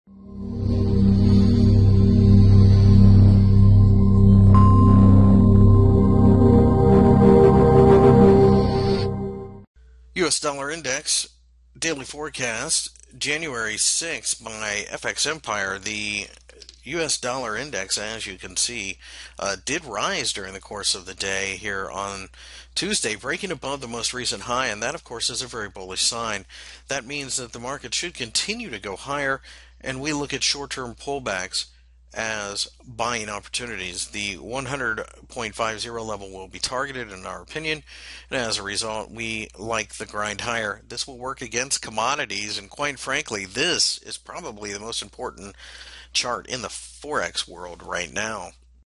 US Dollar Index Technical Analysis for January 6 2016 by FXEmpire.com
- January 6 2016 index daily technical analysis for the US Dollar INDEX. Find more information about Forex and Commodities News Technical Analysis Fundamental Analysis and Brokers on FX Empire